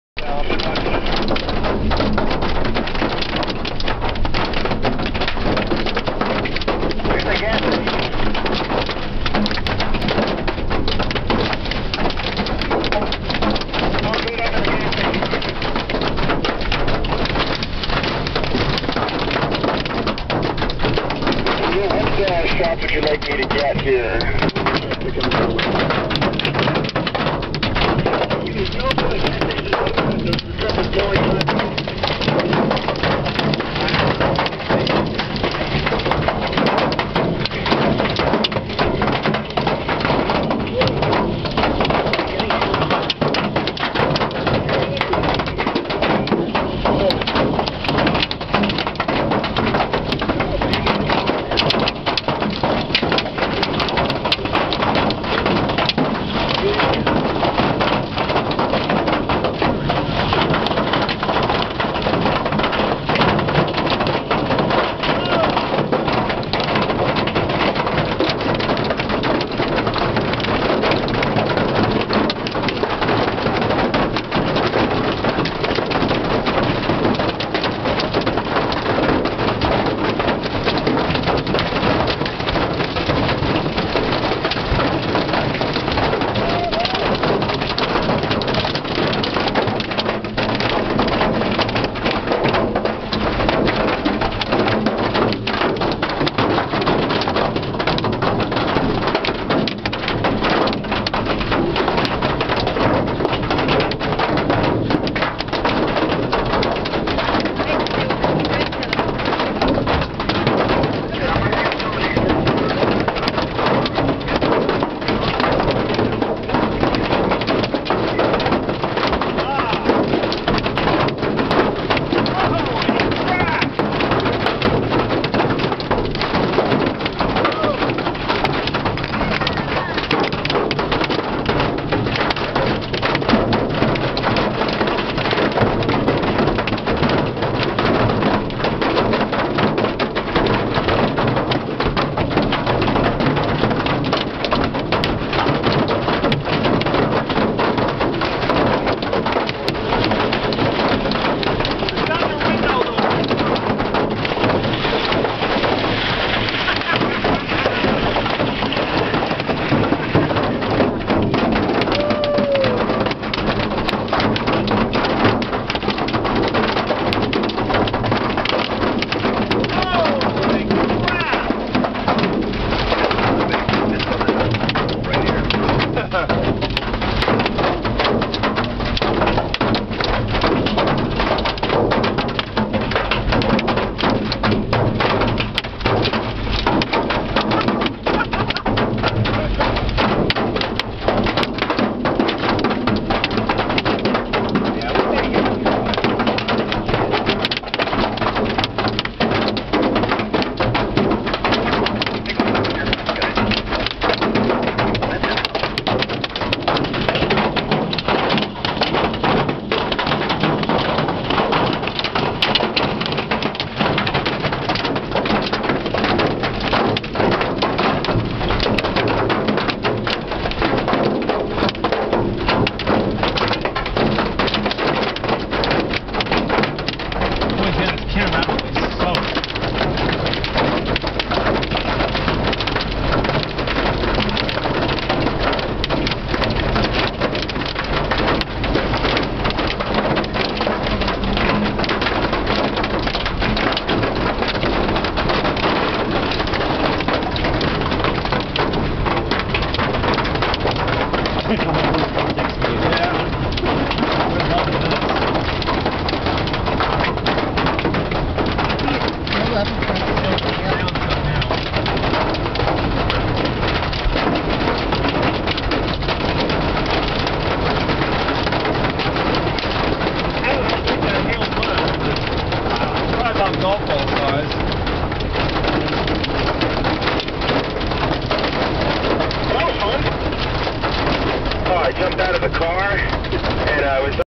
Chased by big hailstorm in Colorado
June 11, 2009: we went chasing some crappy thunderstorms, hoping to see some nice cloudformations. Suddenly the weak storms got more organised and one of them turned into a classic supercell which completely surprised us with golfbal sized hail and gusty winds. It brought some minor damage to our vehicles, which was compensated by the fun we had in the storm.